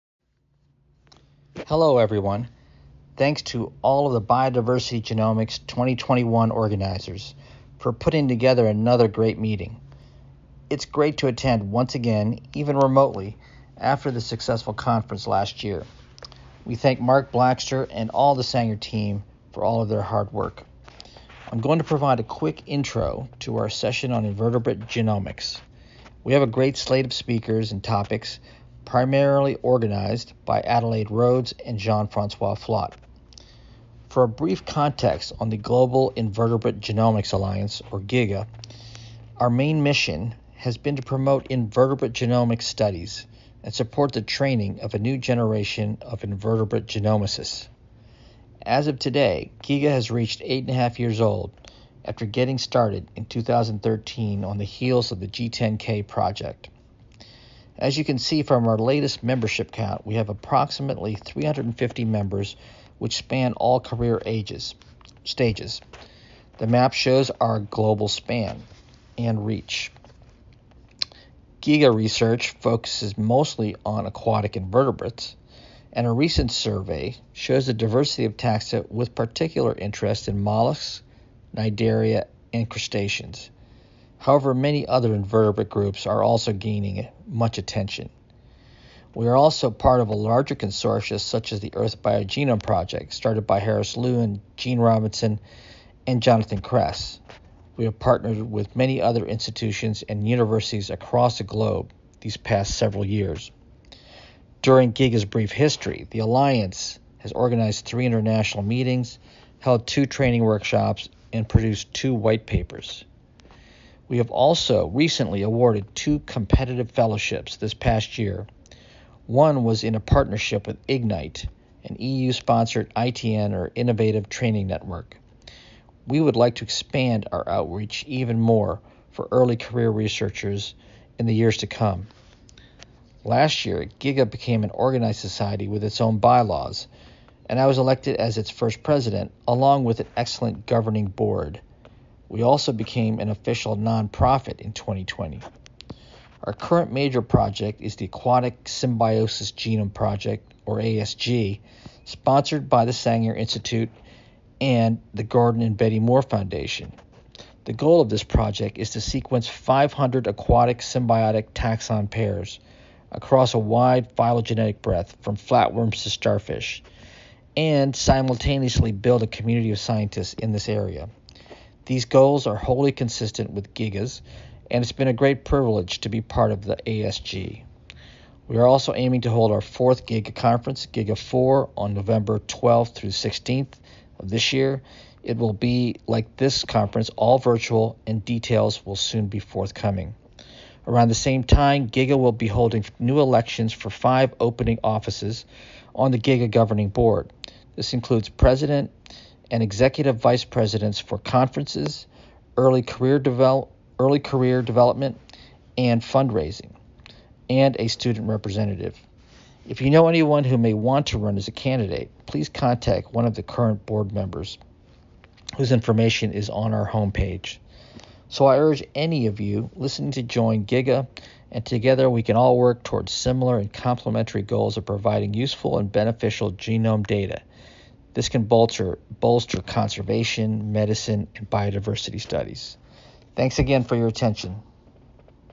Global Invertebrate Genomics Alliance - 2021 Project Summary
Joe Lopez from Nova Southeastern University discusses the Global Invertebrate Genomics Alliance (GIGA).